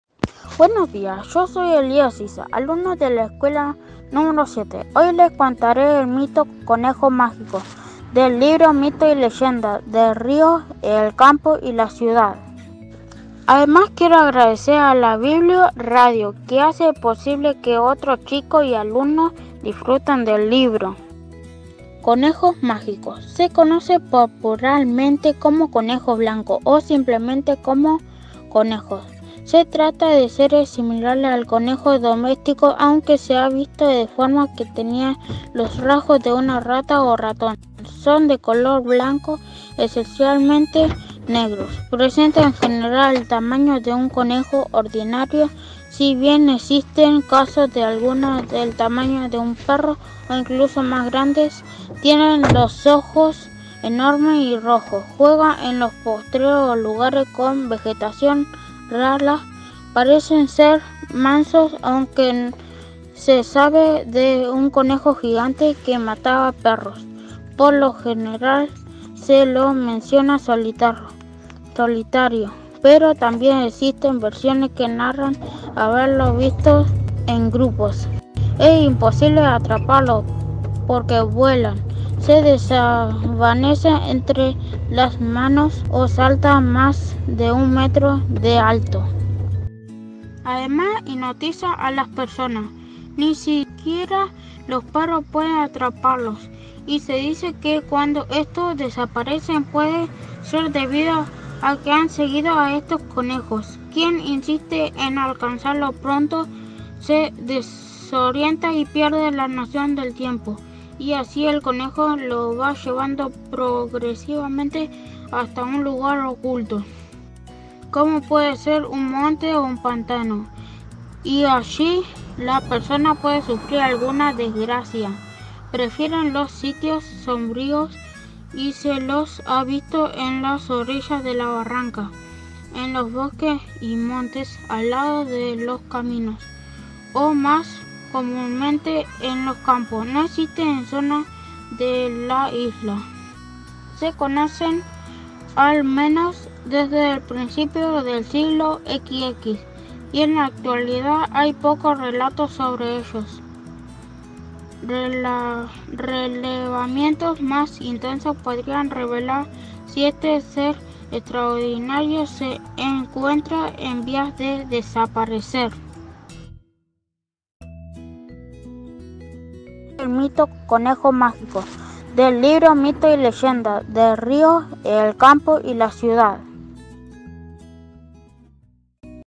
Conejo mágico del libro Mitos y leyendas del río, el campo y la ciudad de Damián Voglino
Narrado por Ellas de la Escuela Nro 7 de Ramallo